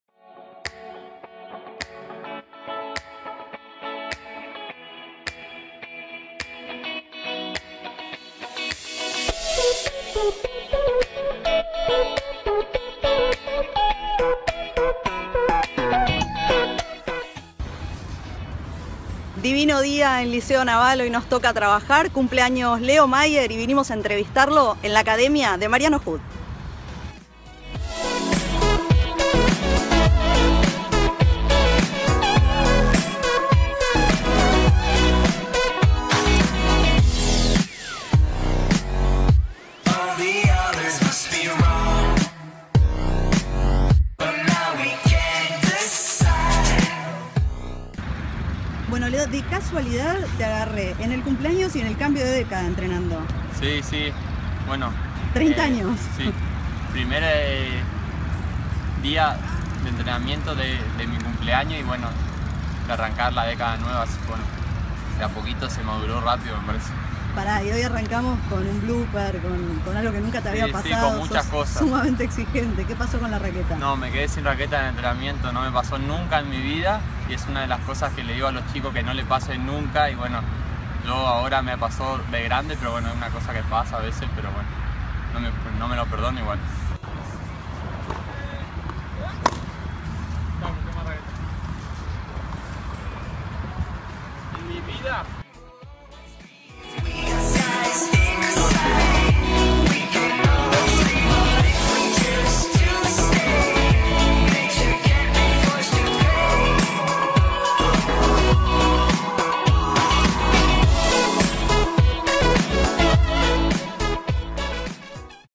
Leo Mayer íntimo con Sole Giménez, parte I
En el cumpleaños 30º del correntino nos fuimos a verlo entrenar y charlamos sobre su momento y un blooper poco común: cortar raqueta durante un entrenamiento y no tener otra."Es la primera vez que me pasa", dijo.